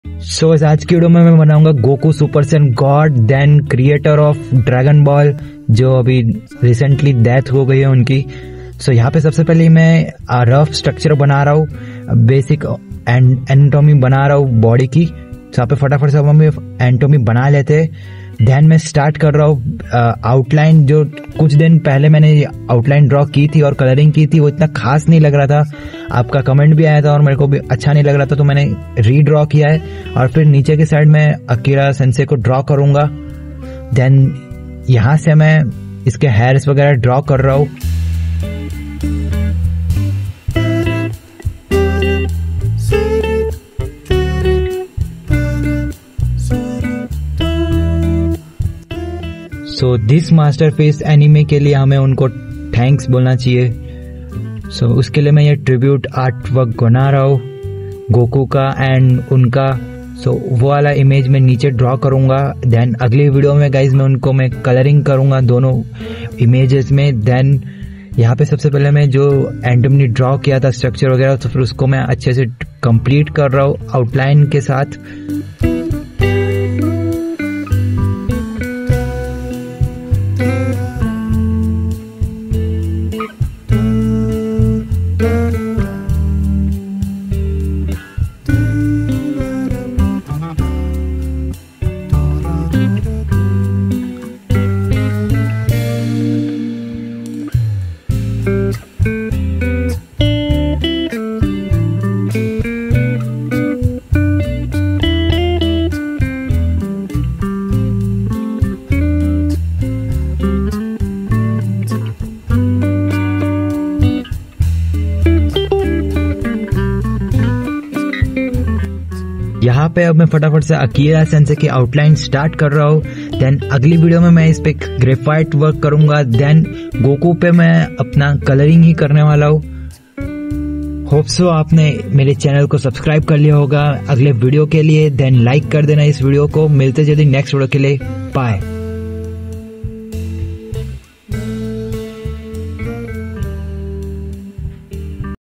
how to draw goku super saiyan god with Akira toriyama goku dragonball #
first Rest In Peace for Akira Toriyama
I hopes guys you like it.
material - brusto A5size paper, and apsara pencil.